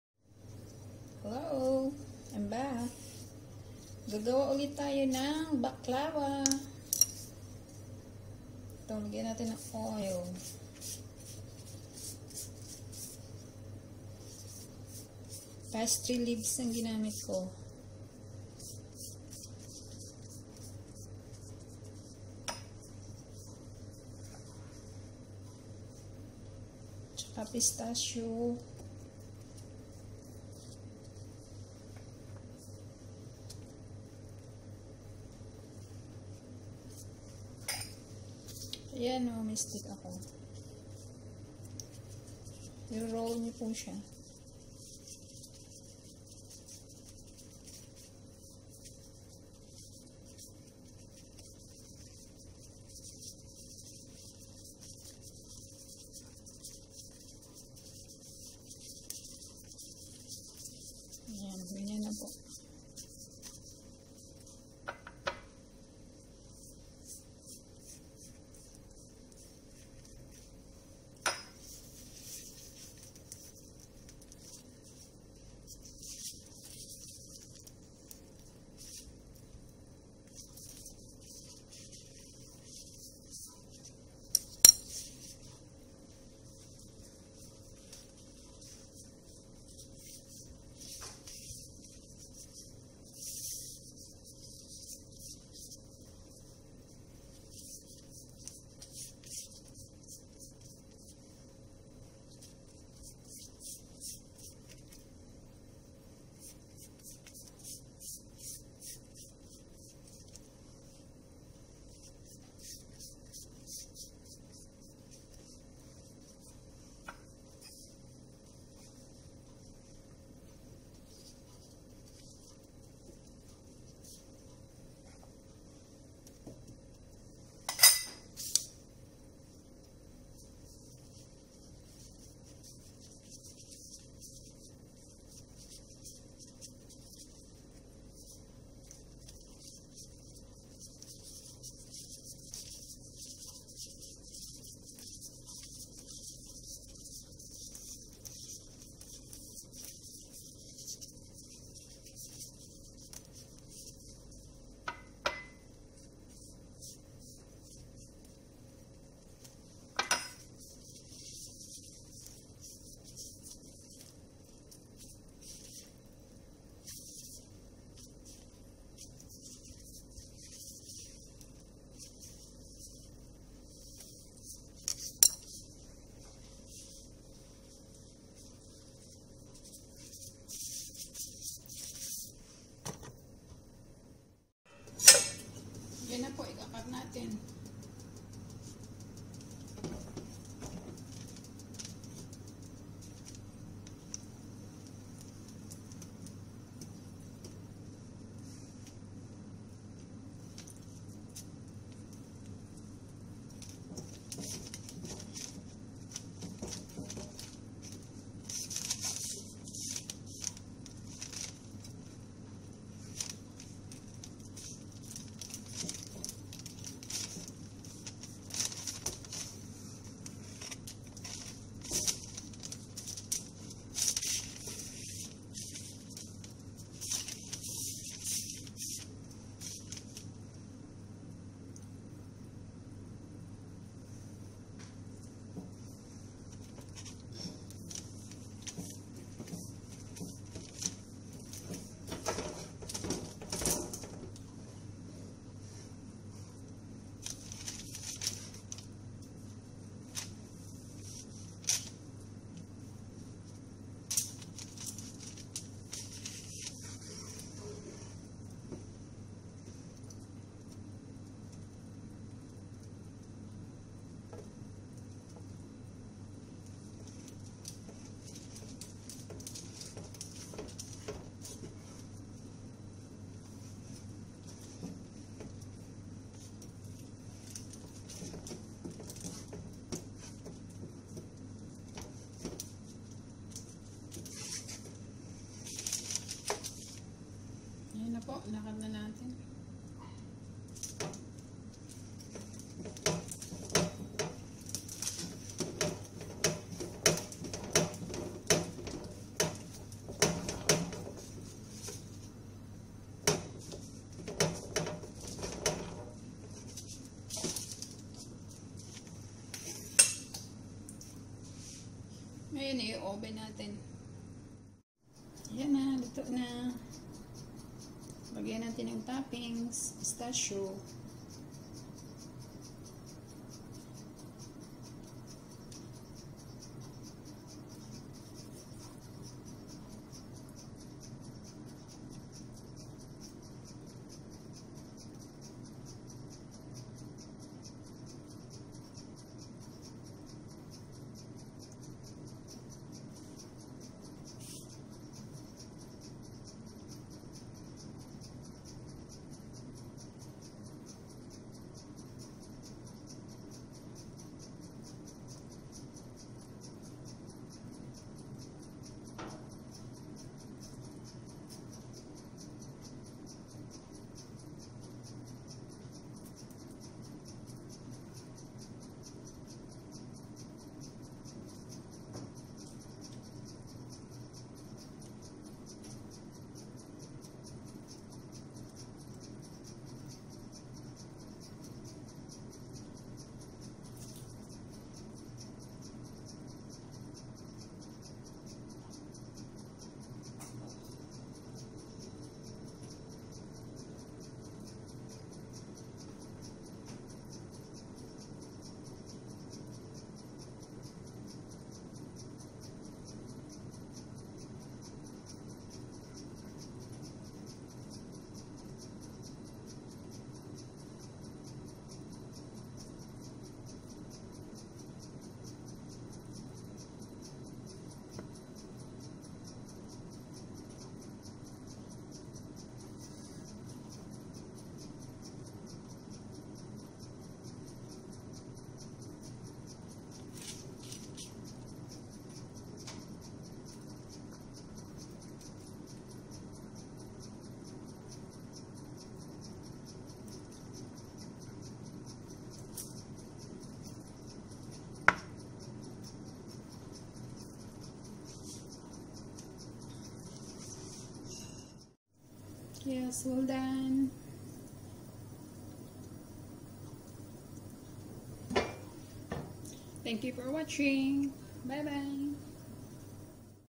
Baklawa Sweet
Ingredients

Pastry leaves
Grated pistachio
Olive oil

Toppings

3 cups white sugar
1 1/2 cup water
1 tbsp lemon juice
2 tbsp rose water